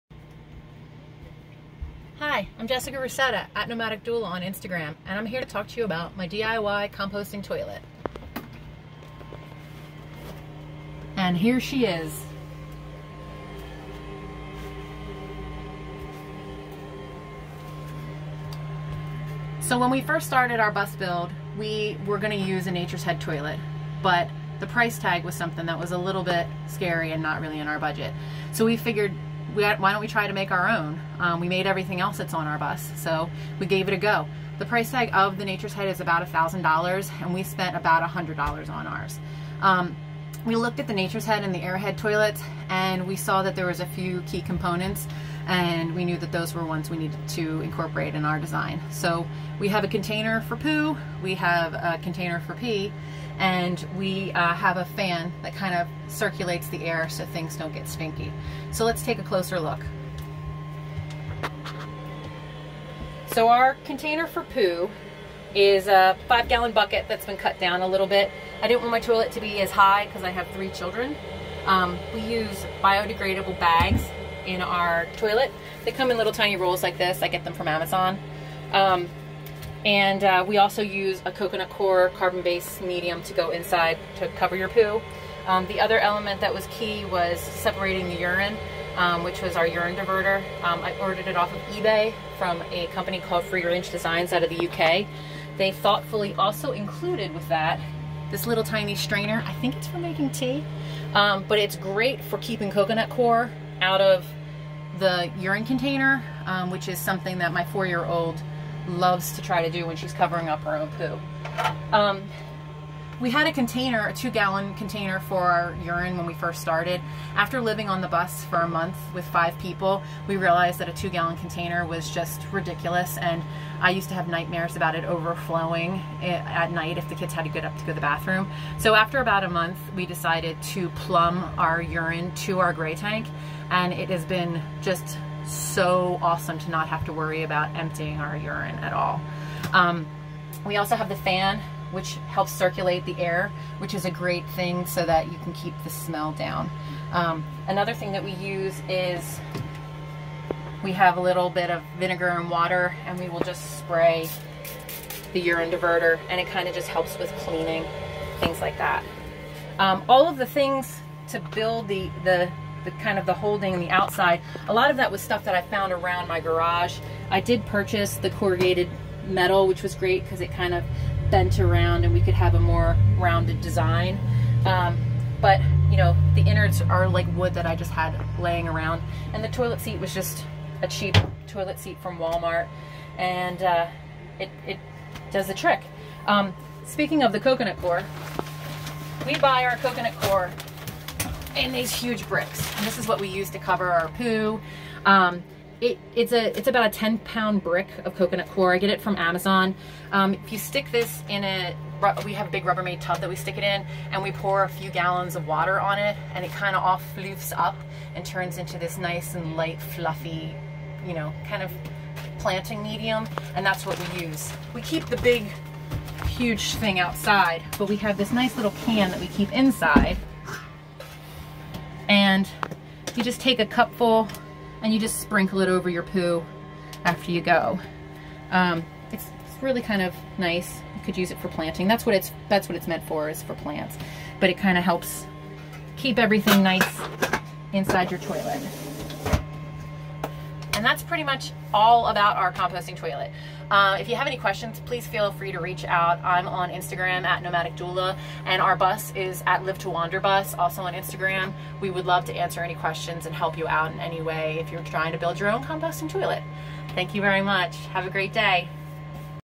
My DIY Composting Toilet
The Online Tiny House Festival asked me to do a tiny talk about my homemade composting toilet.